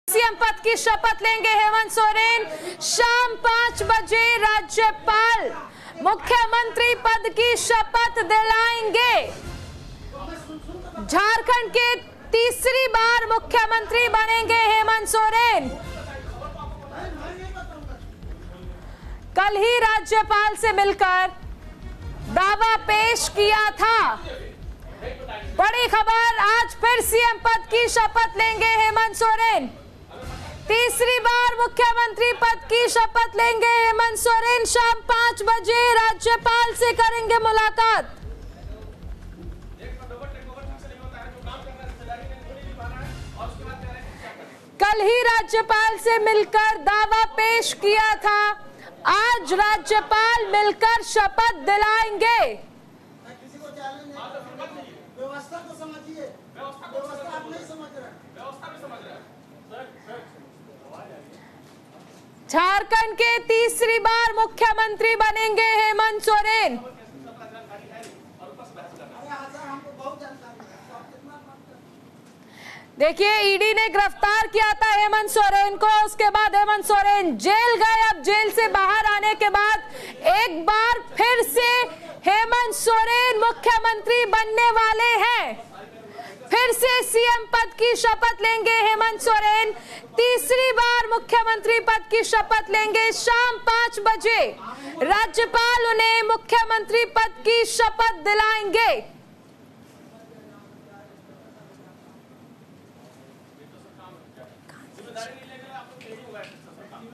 Hemant Soren Oath Ceremony Live: आज फिर सीएम पद की शपथ लेंगे हेमंत सोरेन | Jharkhand
झारखंड के मुख्यमंत्री चंपई सोरेन ने बुधवार को सीएम पद से इस्तीफा दे दिया। जिसके बाद हेमंत सोरेन ने राज्यपाल के समक्ष सरकार बनाने का दावा पेश कर दिया और कई विधायकों का समर्थन पत्र भी सौंपा। वहीं, अब आज हेमंत सोरेन शाम 5 बजे मुख्यमंत्री पद की शपथ लेंगे। शपत ग्रहण समारोह की तैयारी की जा रही है।

Jharkhand Chief Minister Champai Soren resigned from the post of CM on Wednesday. After which Hemant Soren staked claim to form the government before the Governor and also submitted letters of support from many MLAs. At the same time, today Hemant Soren will take oath as Chief Minister at 5 pm. Preparations are being made for the oath taking ceremony.